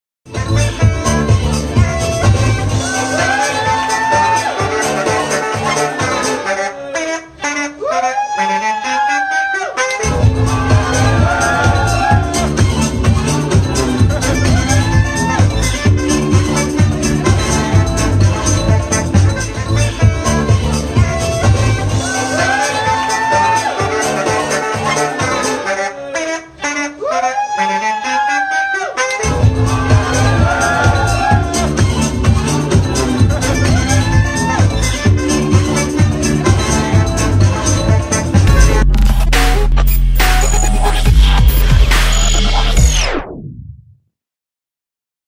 Gece kulübünde saksafonu ile insanları coşturan müzisyen
Gittiği gece kulübüne saksafonunu da getiren adam, dayanamamış ve DJ'ye eşlik etmeye karar vermiş. İlk başta tuhaf gözükse de ortaya güzel bir birliktelik çıkmış.